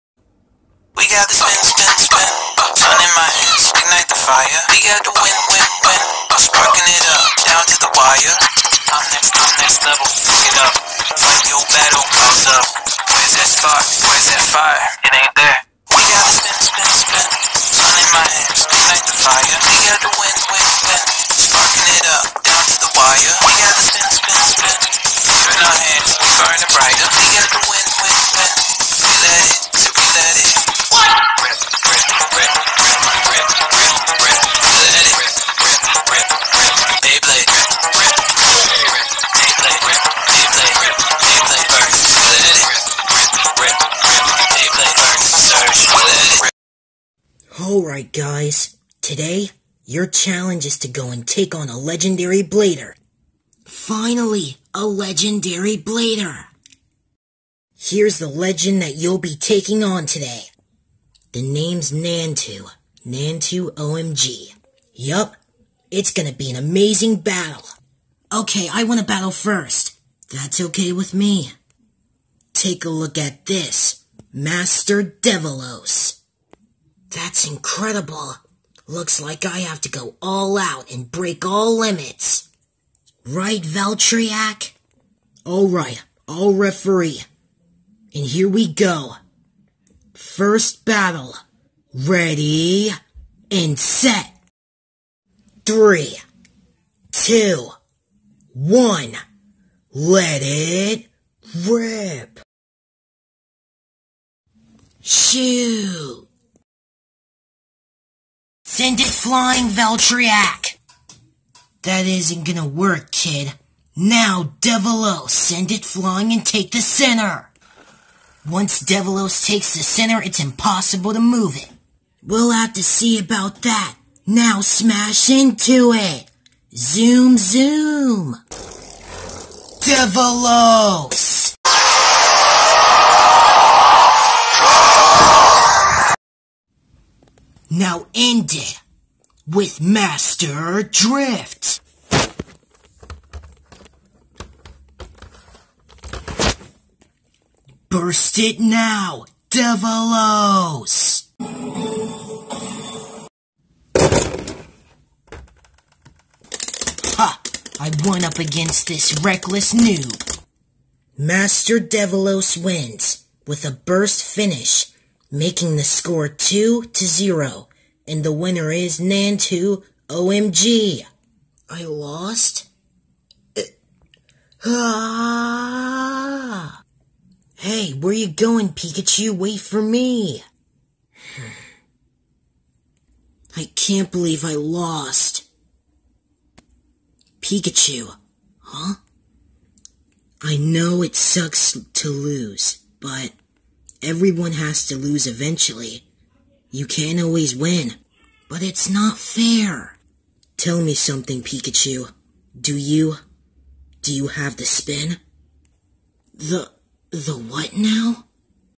Beyblade burst surge plush ep4:master devolos's challenge
the two protagonists meet and battle a new legendary blader